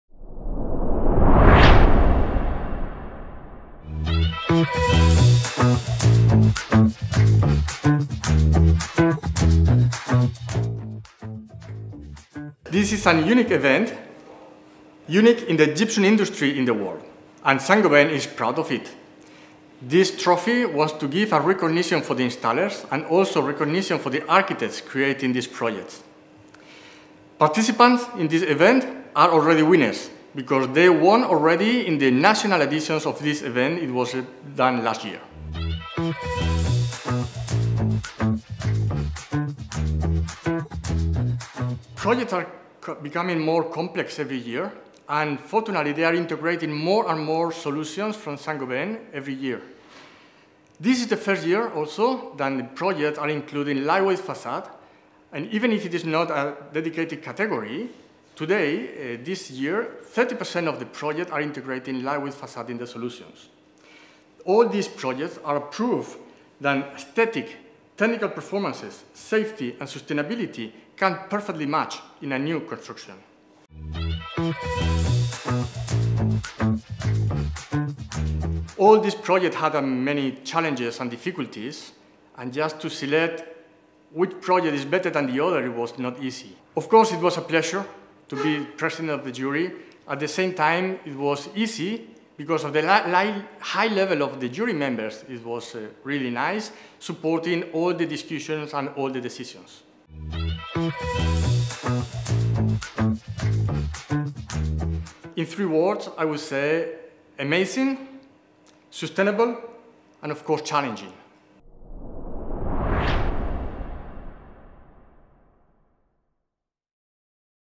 13th Saint-Gobain International Gypsum Trophy – Interview 1
Discover the interview of Fernando Martin, President of the Jury and International Senior Product Manager at Saint-Gobain.